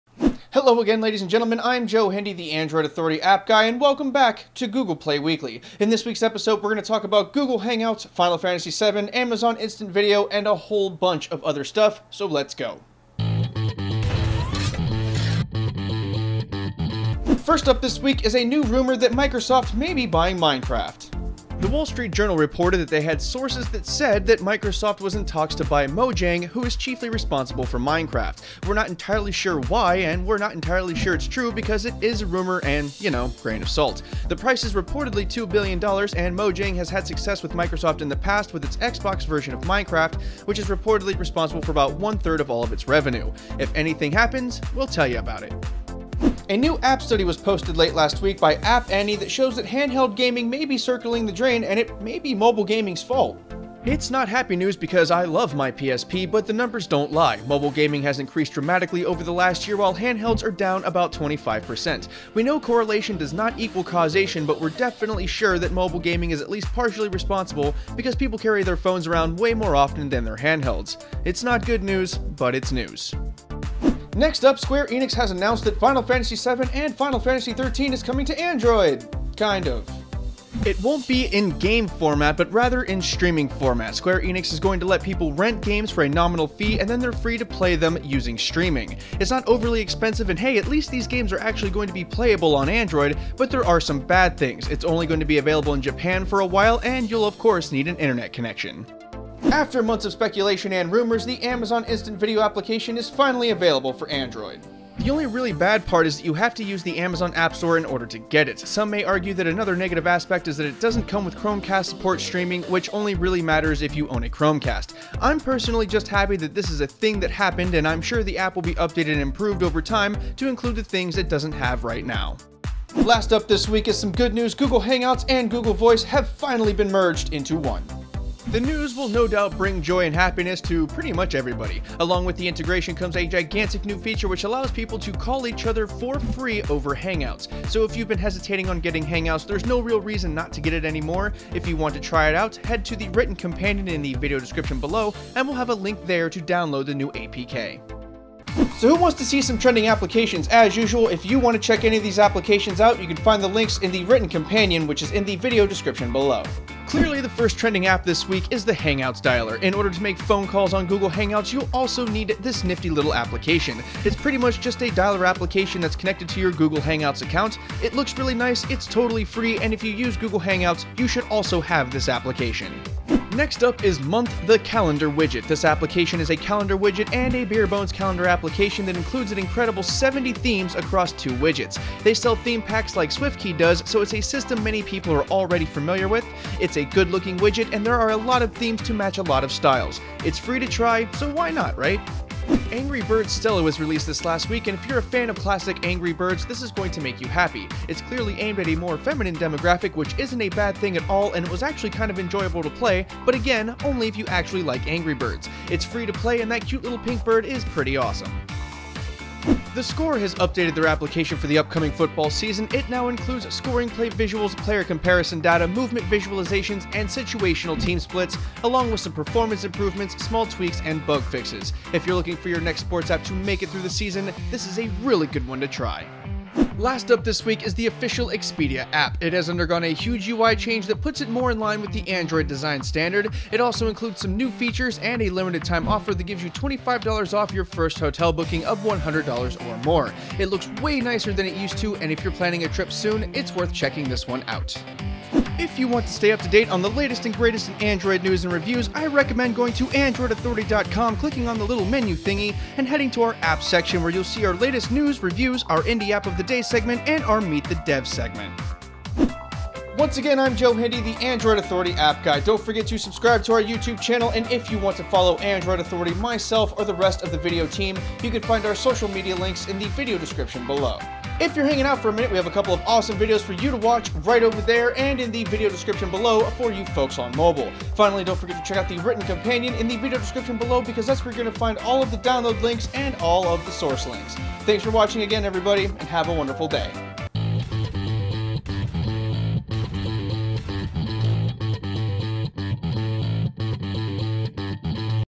Hangouts, Voice merge finally, Amazon Video finally, Final Fantasy 7 finally! - Google Play Weekly
Join us this week as we talk about Amazon Instant Video on Android (finally), Hangouts and Google Voice merging (finally), and Final Fantasy 7 coming to Android (finally!) along with a lot more Android apps news!

Check out the latest Android Authority deals!

Check out the Android Authority community!

Get your Android Authority merch!